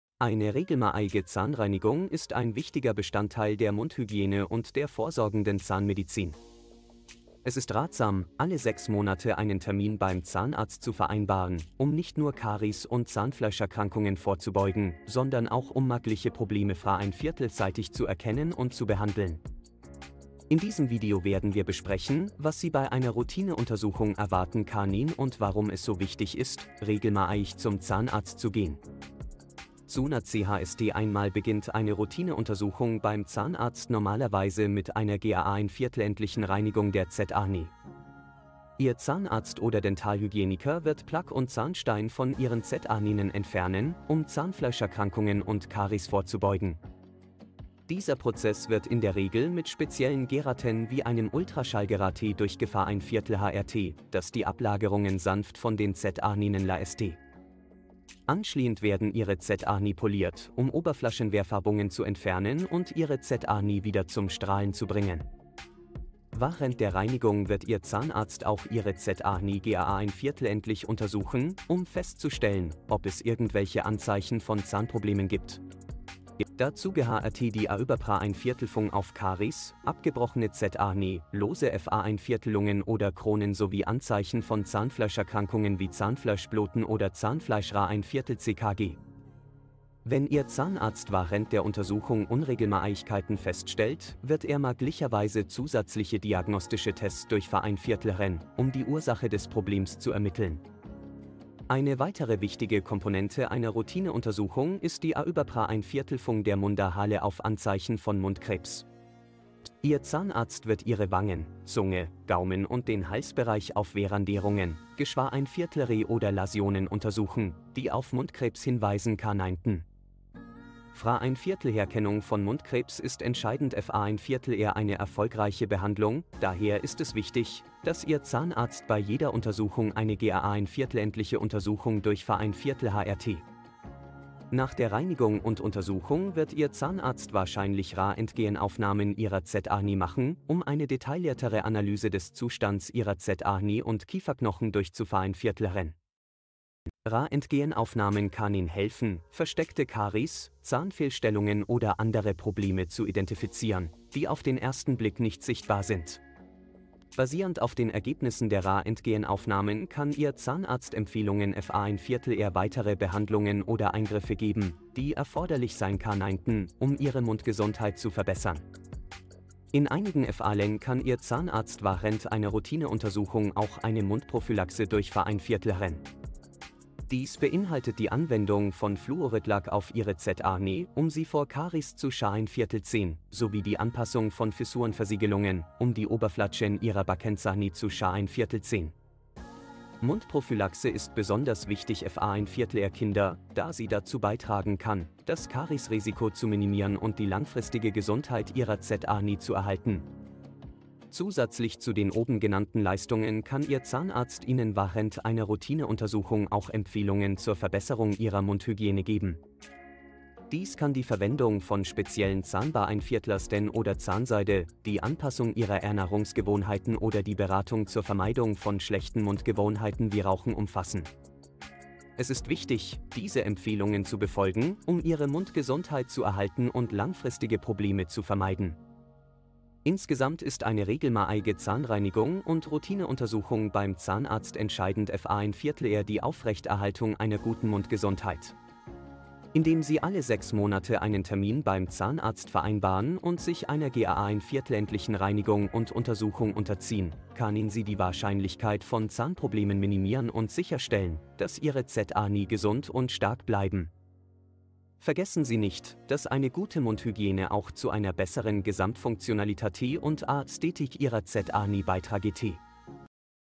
Regelmäßige Zahnreinigung - Wie oft sollte man zum Zahnarzt gehen und warum ist es wichtig?
Kontaktiere uns:
Telegram Eine regelmäßige Zahnreinigung ist ein wichtiger Bestandteil der Mundhygiene und der vorsorgenden Zahnmedizin. Dabei werden Zahnbeläge, die sich trotz regelmäßigem Zähneputzen ansammeln, gründlich entfernt. Dies hilft, Karies und Zahnfleischerkrankungen zu vermeiden und die Gesundheit der Zähne und des Zahnfleisches zu erhalten. Bei der Zahnreinigung werden auch mögliche Verfärbungen durch Kaffee, Tee oder Nikotin entfernt, wodurch die Zähne wieder strahlend weiß werden. Die Behandlung wird in der Regel alle sechs Monate empfohlen, um eine optimale Mundhygiene aufrechtzuerhalten. Sie ist schmerzfrei und dauert in der Regel nicht länger als eine halbe Stunde. Eine regelmäßige Zahnreinigung ist somit ein wichtiger Bestandteil für gesunde und schöne Zähne.